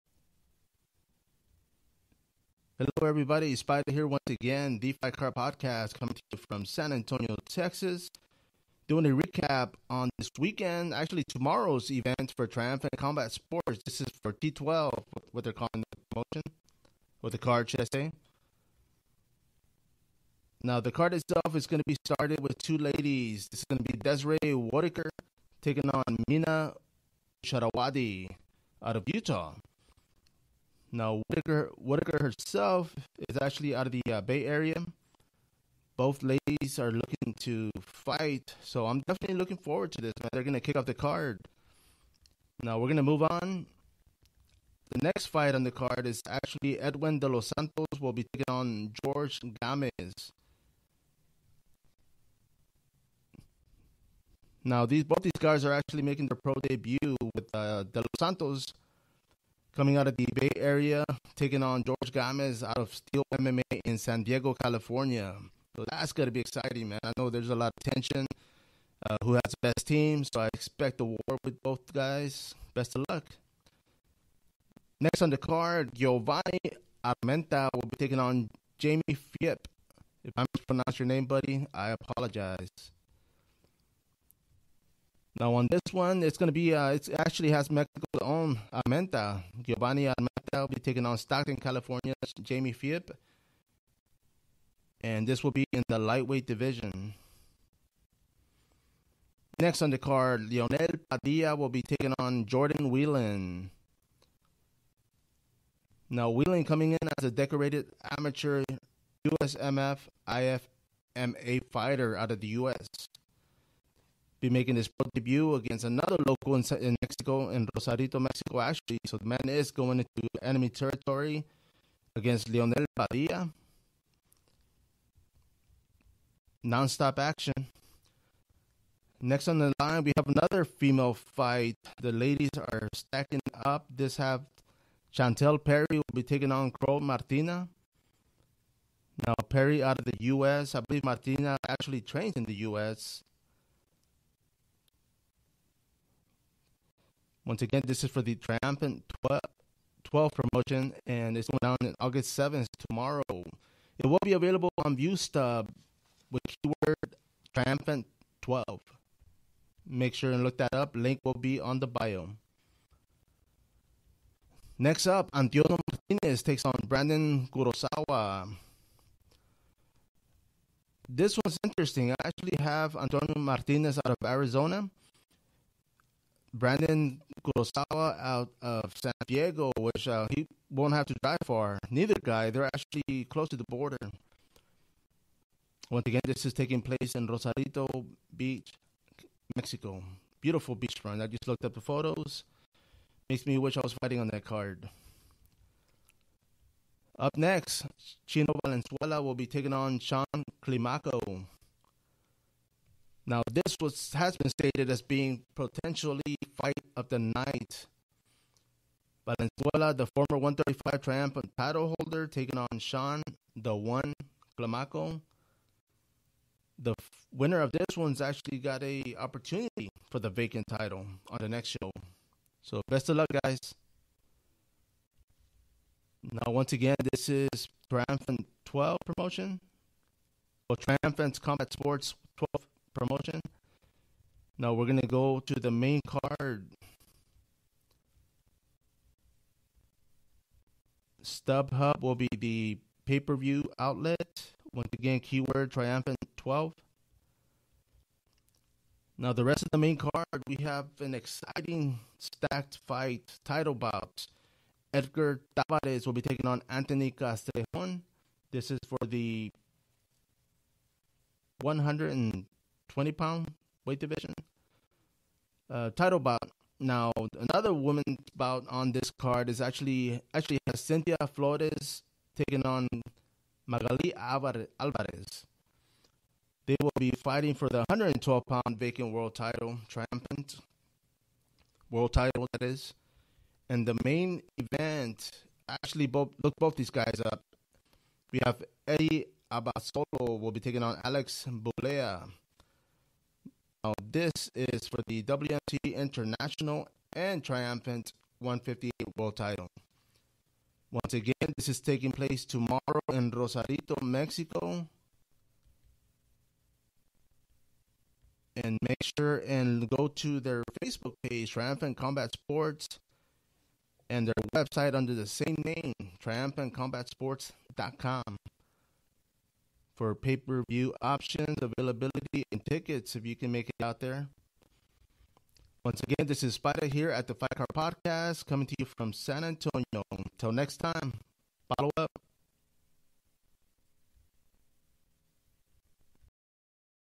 Triumphant 12 (Re-Cap)
Thriumphant Combat Sports is bringign on a night of great Muay Thai action. With fighters from all spects of the fight game. A few making their Pro debut but coming in with tons of great amateur experiance. Tahnk you for listening and follow up!